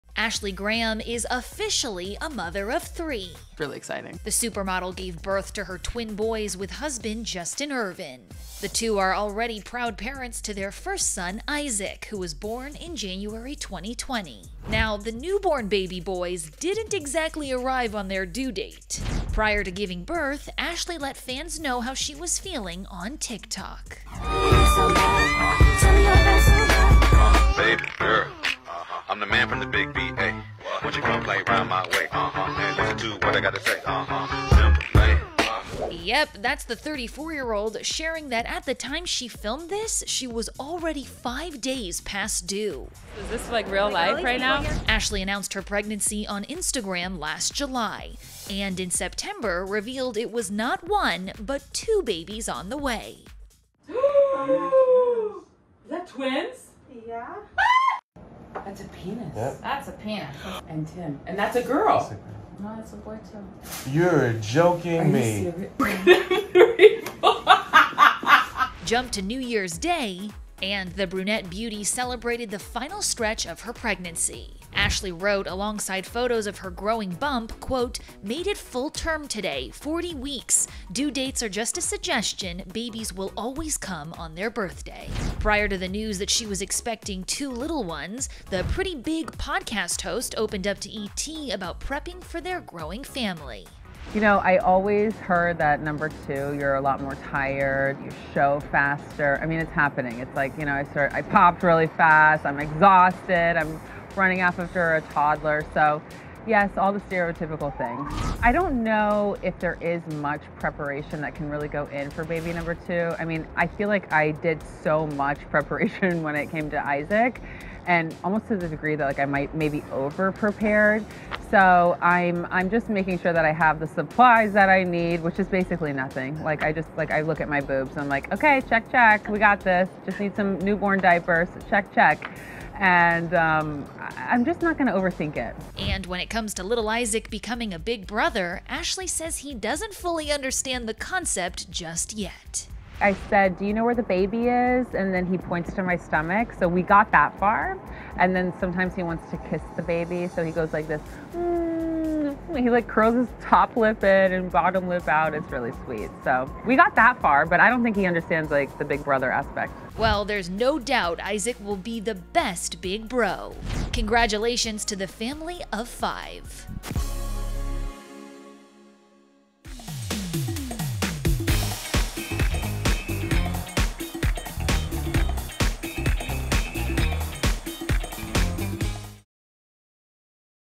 Ashley Graham Gives Birth to TWINS!
Congratulations to Ashley Graham! The 34-year-old supermodel gave birth to her twin boys with her husband, Justin Ervin. The two are already proud parents to their son, Issac Ervin, who was born in January 2020. When ET caught up with Ashley prior to knowing two were on the way, she shared how she was preparing for their growing family.